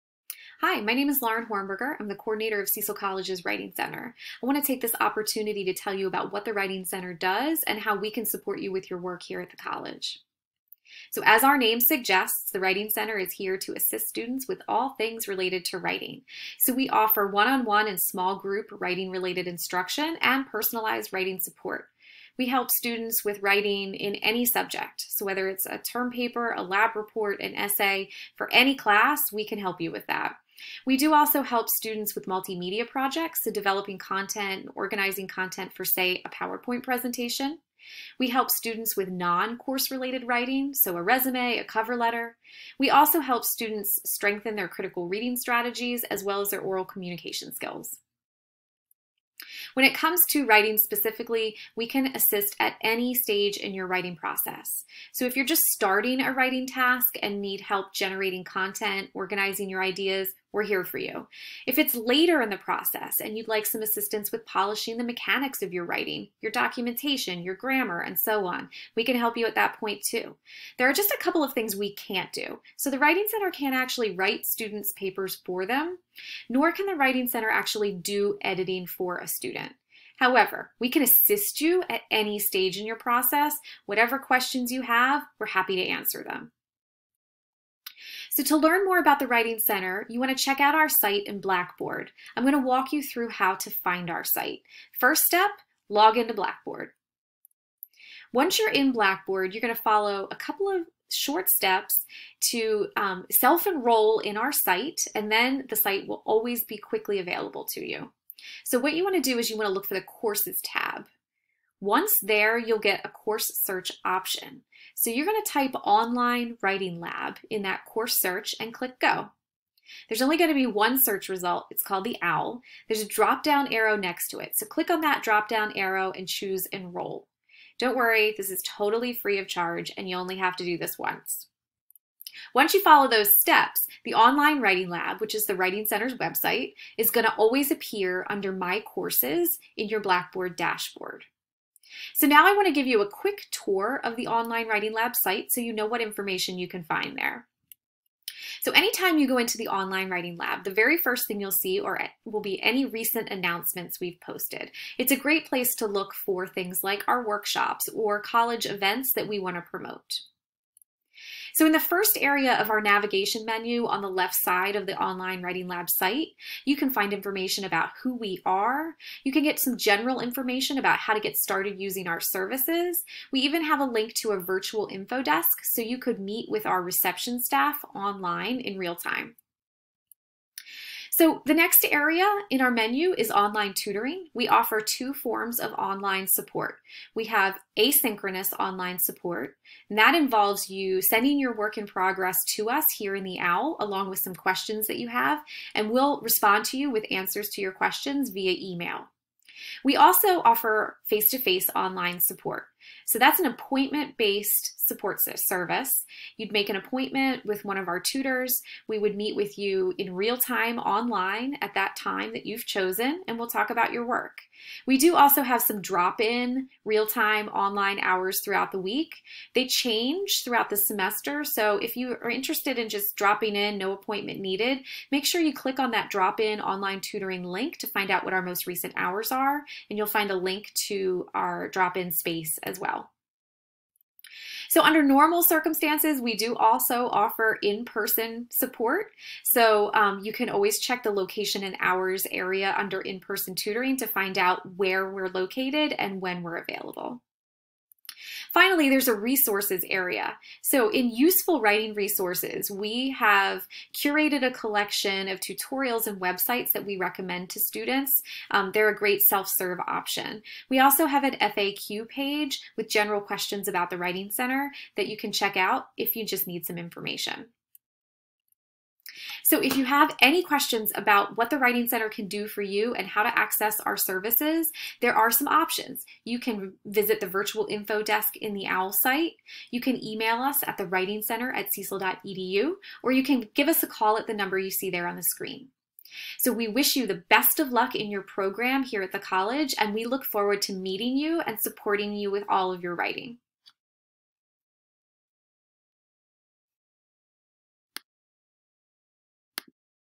Cecil College Writing Center - 2021
A review of the services and resources offered by the Cecil College Writing Center.